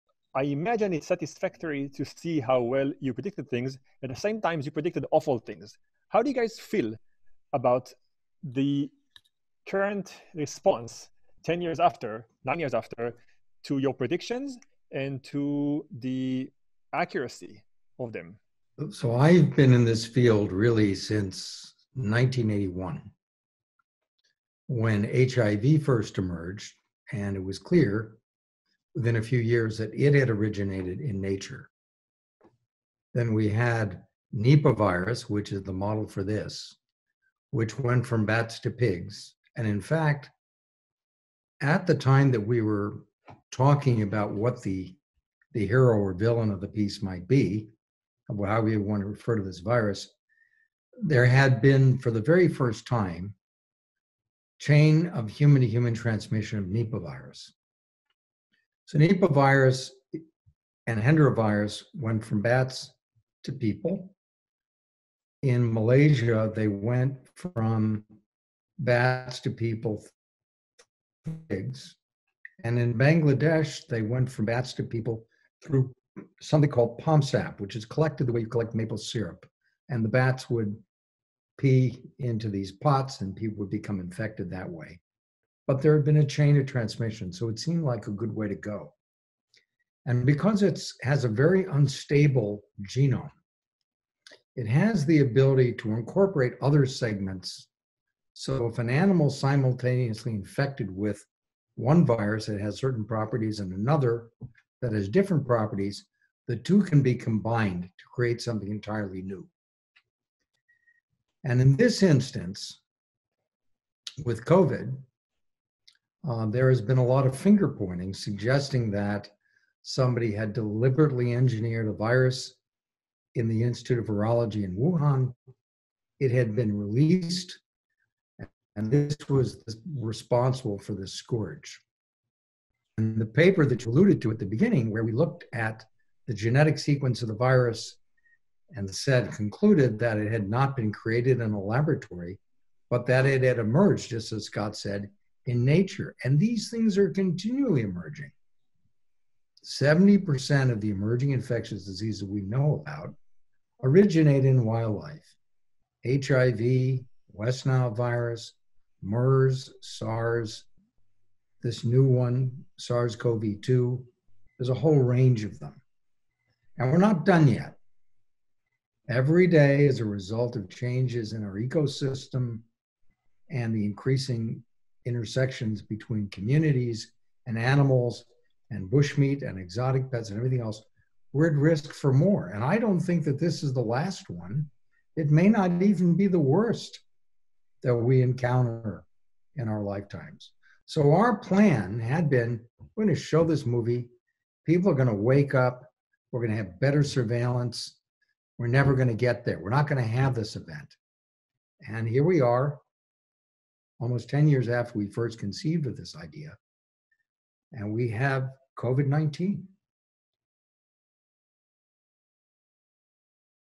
Dr. Ian Lipkin talks about COVID-19 pandemic almost a decade after the release of CONTAGION
In June 2020, an AFI Conservatory seminar with CONTAGION screenwriter Scott Z. Burns and CONTAGION science advisor Dr. Ian Lipkin -- a renowned epidemiologist who has been called a "master virus hunter" for his speed and innovative methods of identifying new viruses. Moderated by AFI Sloan Faculty Dr. Moran Cerf.